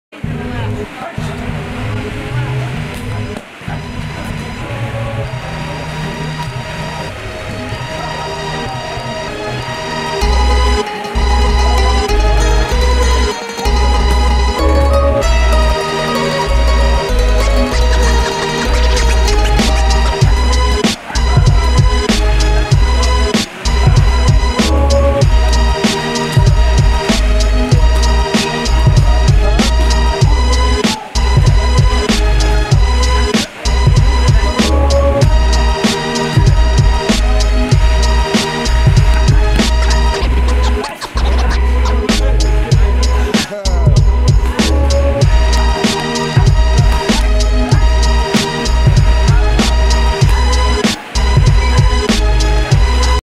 Nieky Holzken training Lethwei with Dave Leduc
Kickboxing Superstar Nieky Holzken training headbutt combos with Lethwei World Champion Dave Leduc.